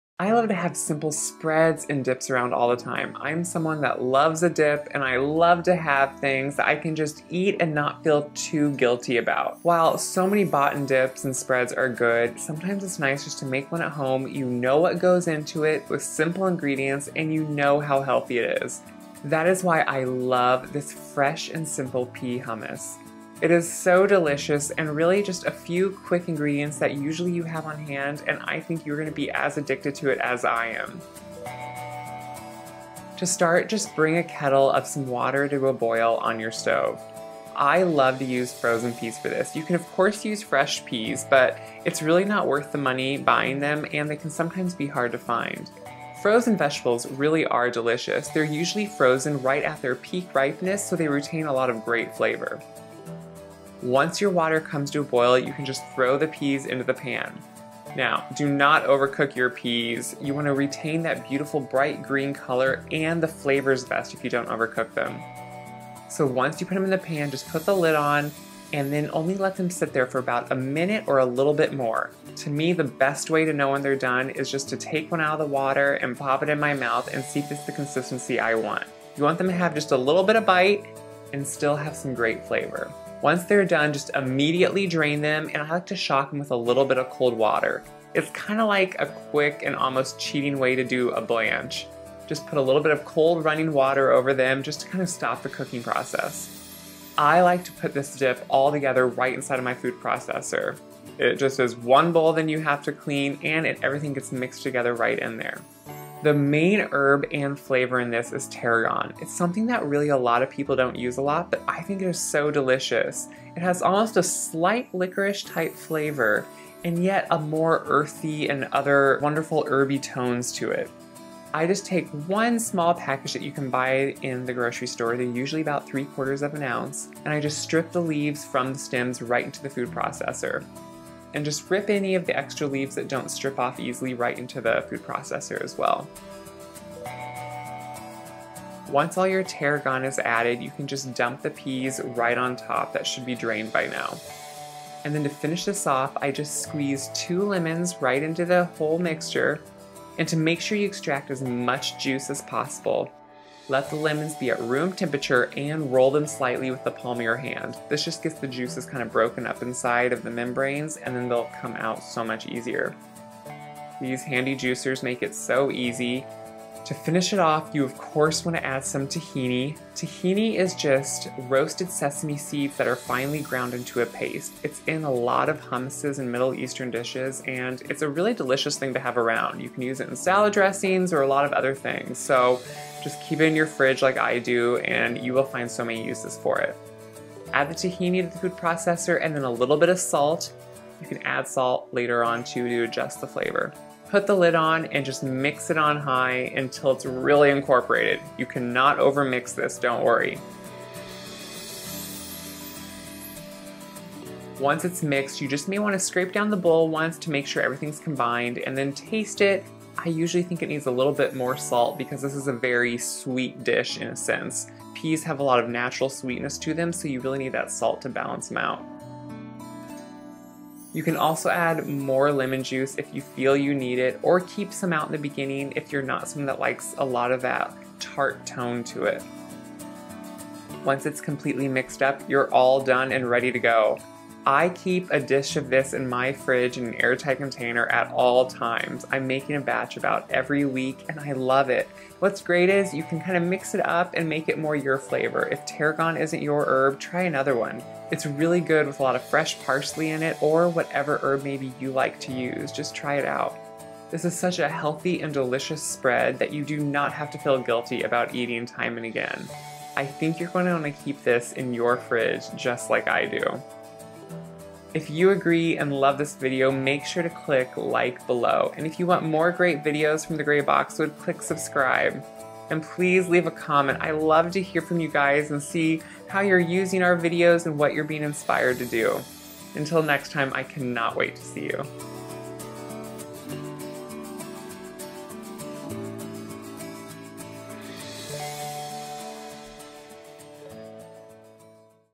Pea Hummus
If you're looking for a traditional hummus recipe, this recipe isn't for you. This is a pea hummus that's slightly sweet and packed with tons of creamy, delicious flavor! Whether it's eaten with vegetables, crackers, or chips, it's a healthy snack that you're sure to make time after time!

INGREDIENTS

- 5 cups frozen peas
- 1/4 cup tahini (roasted sesame seed spread)
- 0.37-ounce package (1/4 cup) fresh tarragon leaves
- juice of two lemons
- 1-2 teaspoons salt

INSTRUCTIONS

Bring 3 quarts of water to a boil in a large kettle. Once boiling, add the frozen peas. Cover and let sit for 1-3 minutes until the peas are just tender. Drain immediately and rinse with cold water. Continue to let drain.

Add tarragon leaves to a food processor followed by drained peas, lemon juice, tahini, and salt. Process until smooth. Scrape down the sides of the processor and mix again until all lumps are combined and everything is evenly incorporated.

If you feel the spread is too thick, while the processor is running, add a few tablespoons of olive oil. This will thin the spread to the consistency you like.

Season with more salt if needed. Peas are fairly sweet and can take more seasoning than you may think at first!